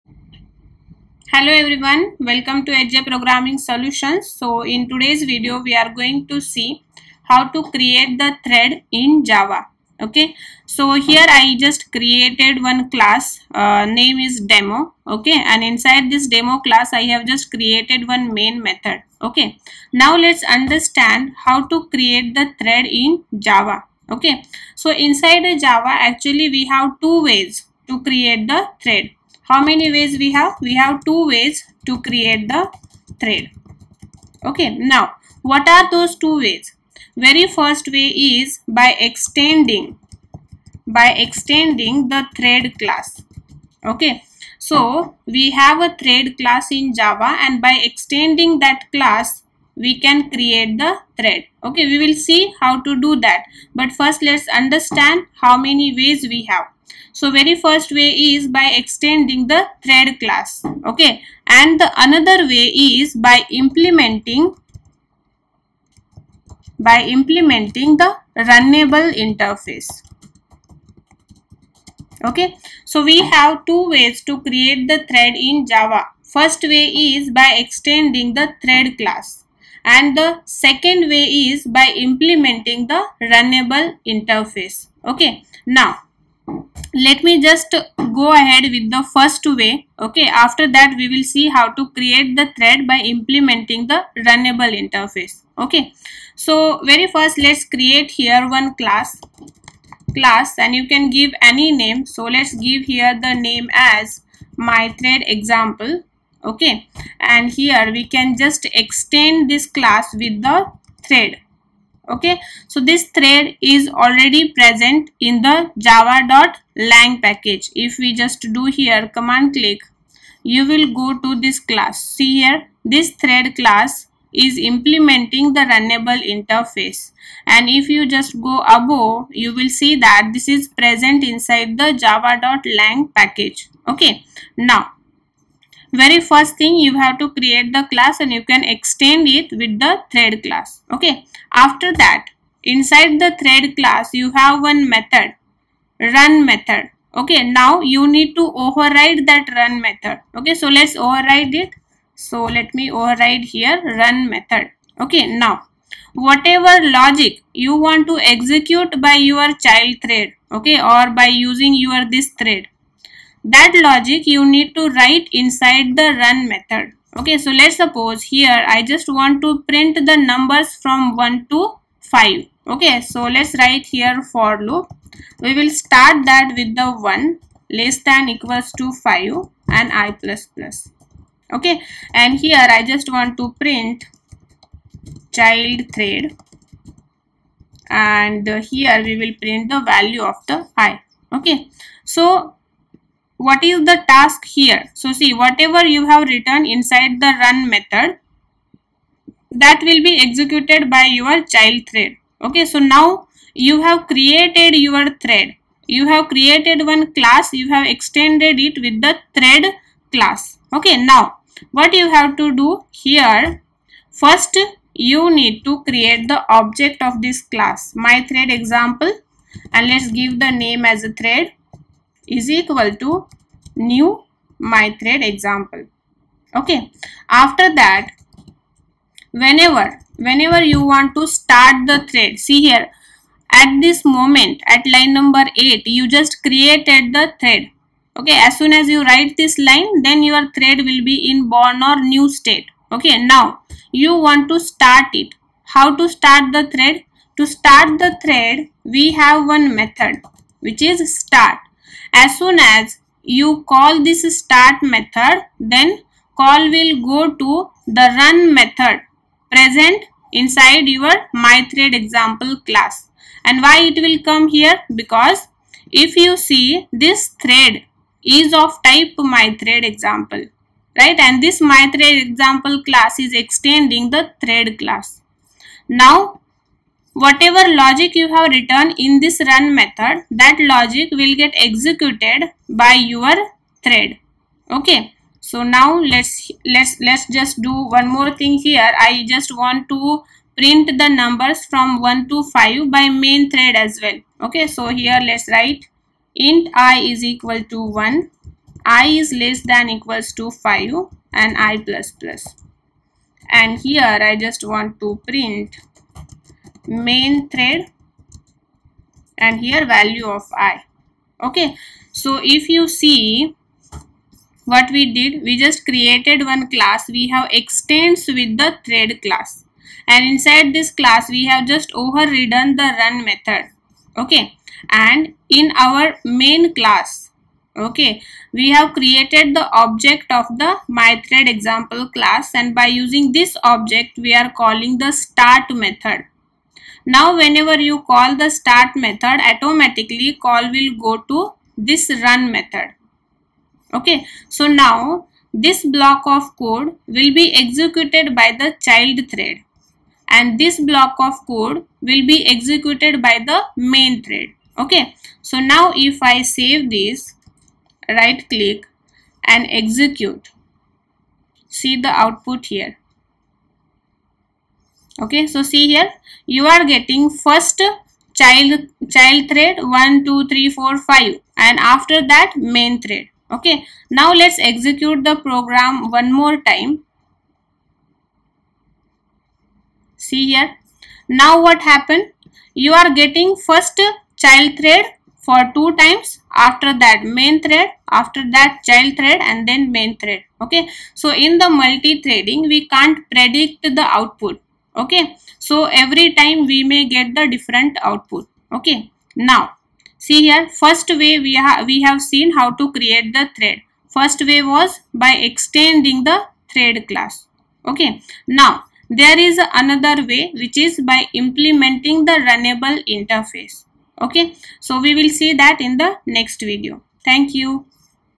How to create Thread in Java by extending Thread class ?
What is Thread ?
How many ways we can create Thread in Java ?
What is threading in Java?
How can we create a thread in Java?
Which is best way to create thread in Java?
When to create threads in Java?
What is thread in Java with example?

Note : This video is purely meant for the educational purpose. There may be slight mistake please take decision based on your own research.